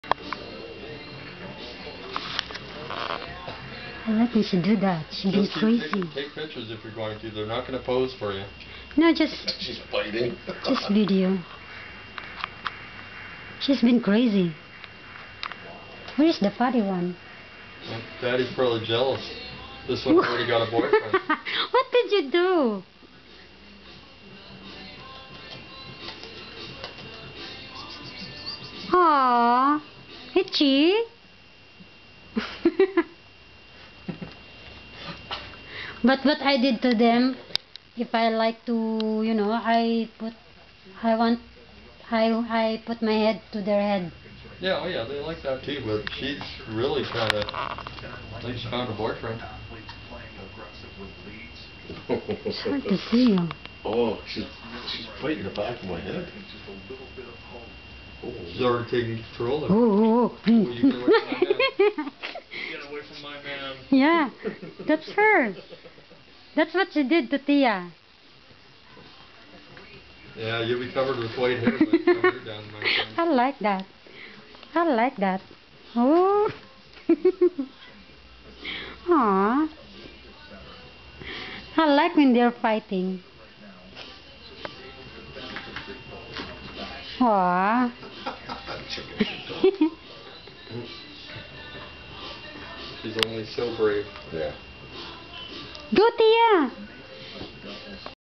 Itchy The Cat Getting Wild of Richard Scent (Aug.31, 2014)
She's kinda crazy about scents..And she's fighting always to Tia..